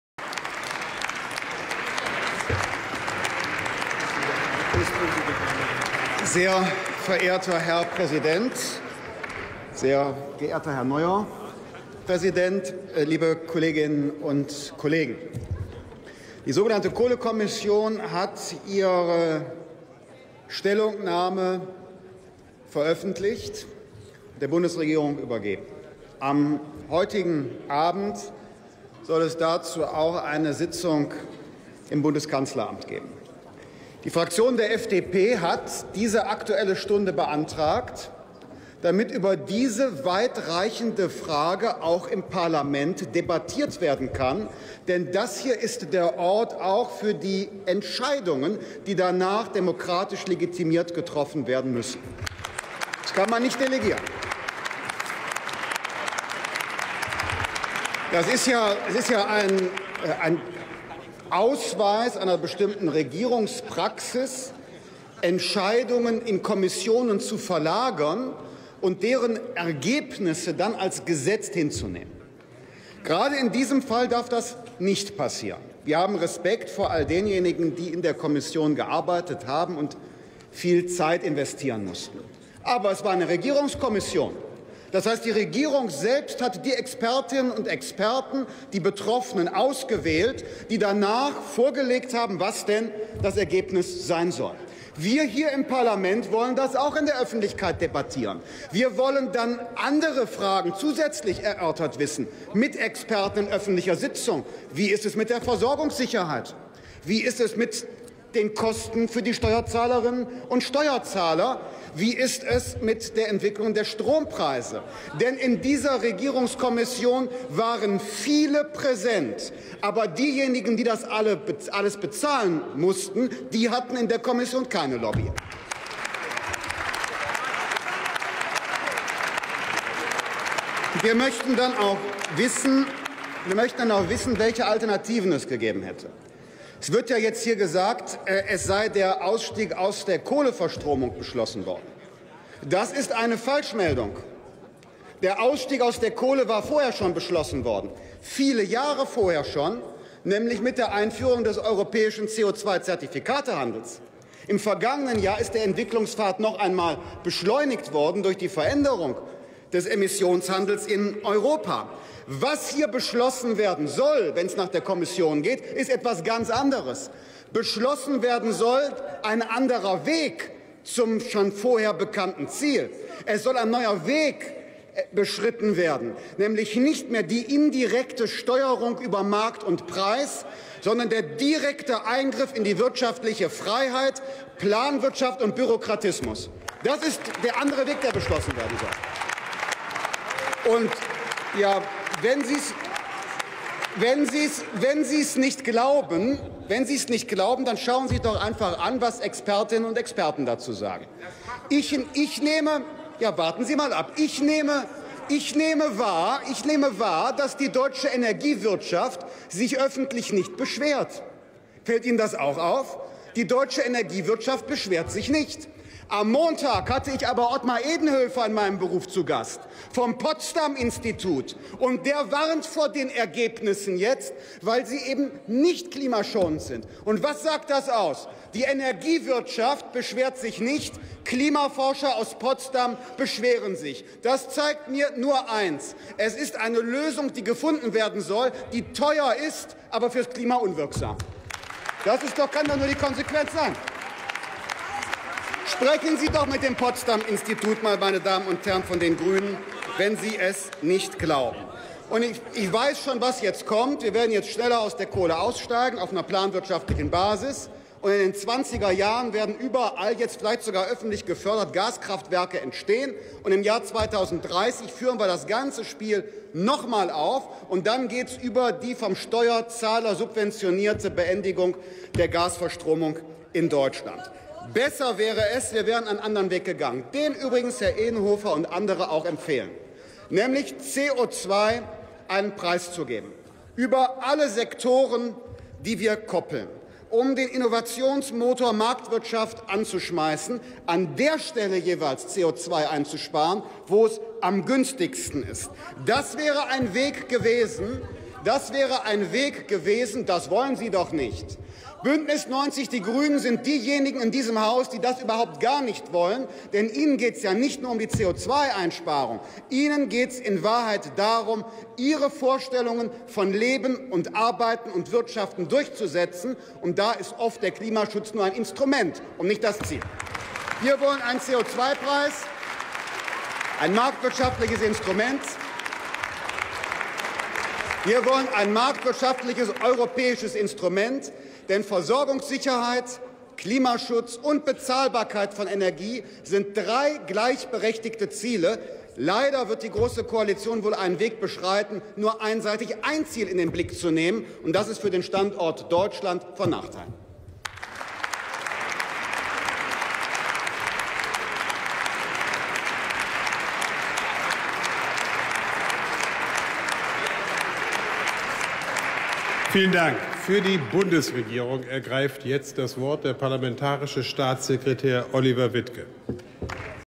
Christian Lindner zur Kohlekommission!
Rede von Christian Lindner am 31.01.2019 im Bundestag.